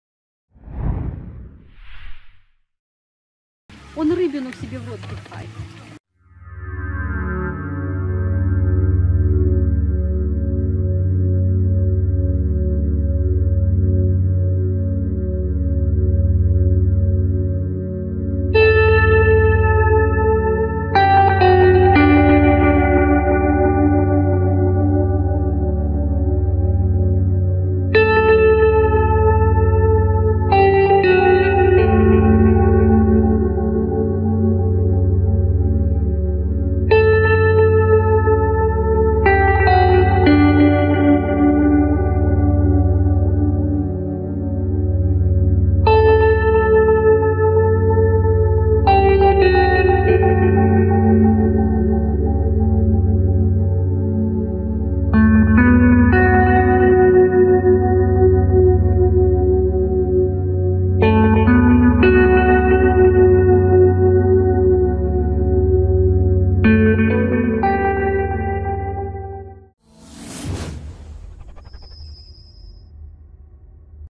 Уж ест рыбу
Лето… Отпуска… Рыбалка… С детства с отвращением и боязнью отношусь к ужам и змеям. Но тут любопытство преодолело страх. Скорее всего Уж пытается впихнуть в себя какую-то рыбешку.